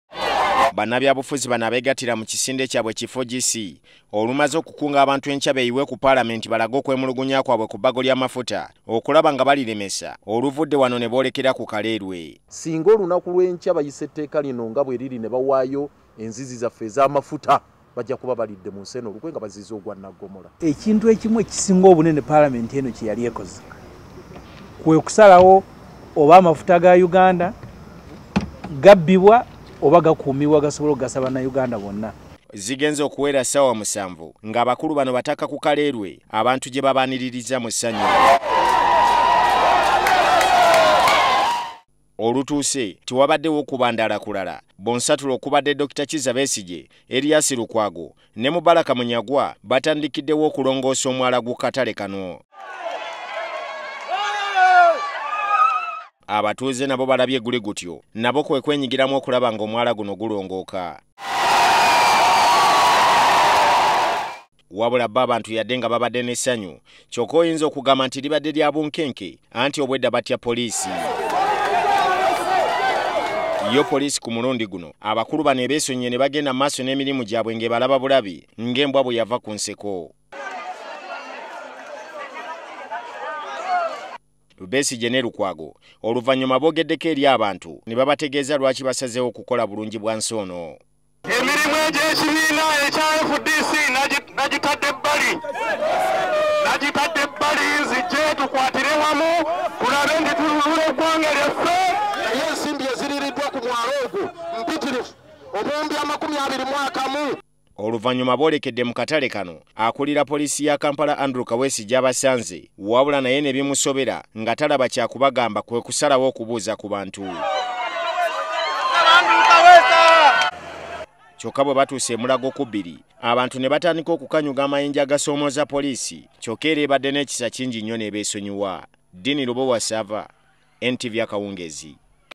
Amafuta: Aba 4GC beegasse ku babaka.
Bannabyabufuzi abeegatira mu kisinde kya 4GC olwa leero beekozemu omulimu ne bakola bulungi bwa nsi mu bitundu by'oku kaleerwe, gyebagogoledde omwala oguyita mu katale akali mu kitundu ekyo kko n'okulambula abakakoleramu. Bano era basoose okubaako n'ebigambo byebateegezeza bannamawulire wali ku kitebe kyabwe ekiri ku Katonga road, nga beemulugunya ku bbago ly'amafuta n'engeri gyebalowoza okuliremesaamu.